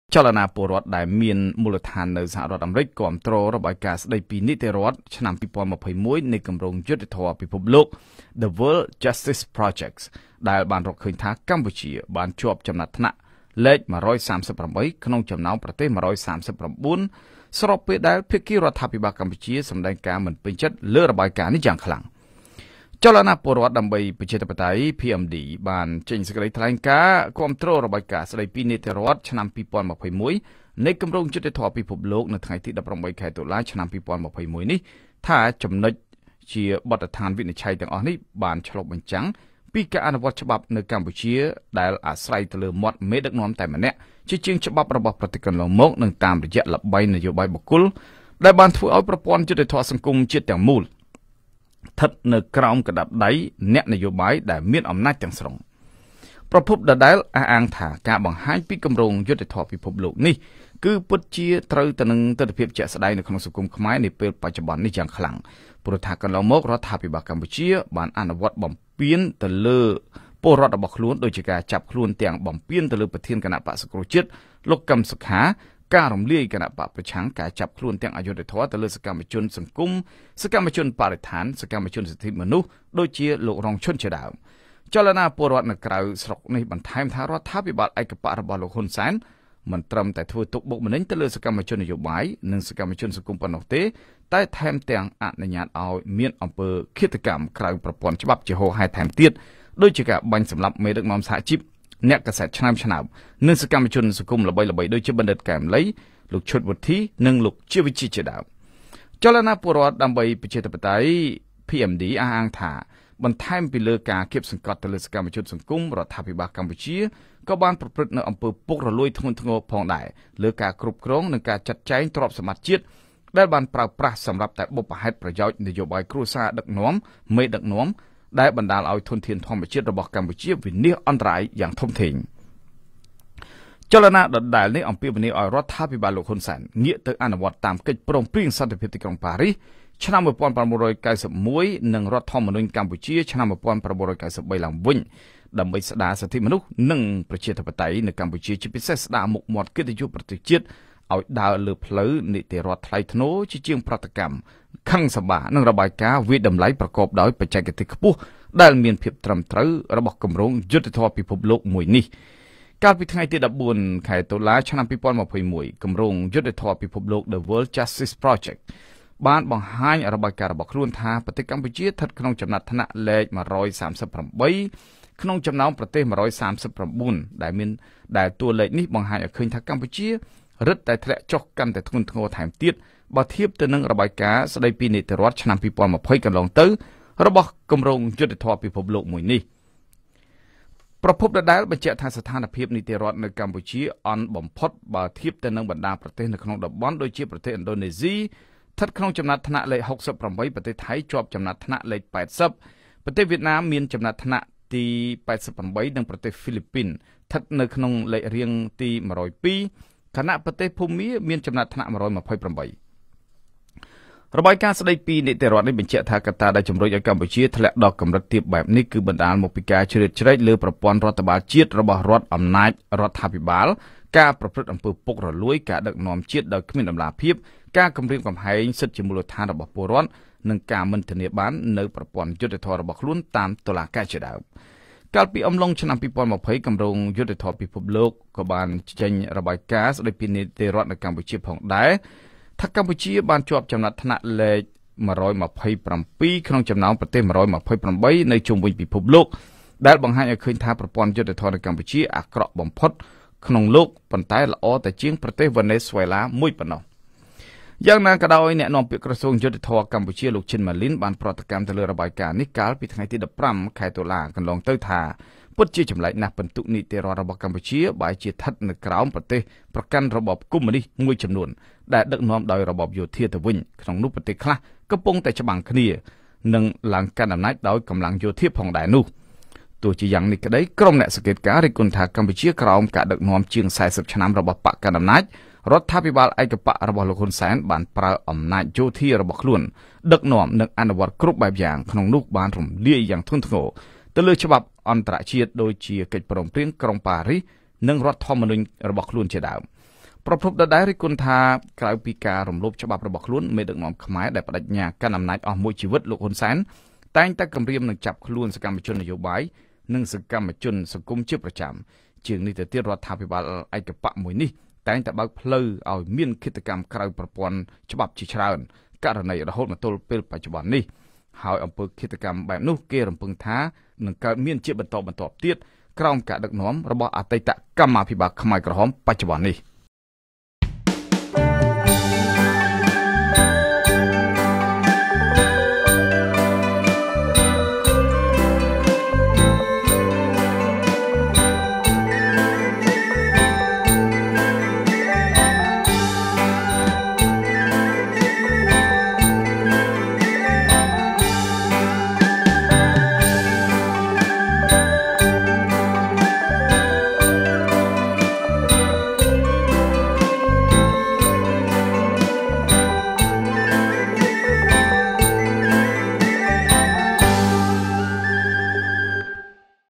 PMD គាំទ្ររបាយការណ៍គម្រោងយុត្តិធម៌ពិភពលោកដែលបង្ហាញថា នីតិរដ្ឋរបស់កម្ពុជាអន់ថយ
ចលនាពលរដ្ឋដែលមានមូលដ្ឋាននៅសហរដ្ឋអាមេរិក គាំទ្ររបាយការណ៍ ស្ដីពីនីតិរដ្ឋ ឆ្នាំ២០២១ នៃគម្រោងយុត្តិធម៌ពិភពលោក The World Justice Project ដែលរកឃើញថា កម្ពុជា បានជាប់ចំណាត់ថ្នាក់លេខទី១៣៨ ក្នុងចំណោមប្រទេស១៣៩ ស្របពេលដែលភាគីរដ្ឋាភិបាលកម្ពុជា សម្ដែងការមិនពេញចិត្តលើរបាយការណ៍នេះយ៉ាងខ្លាំង។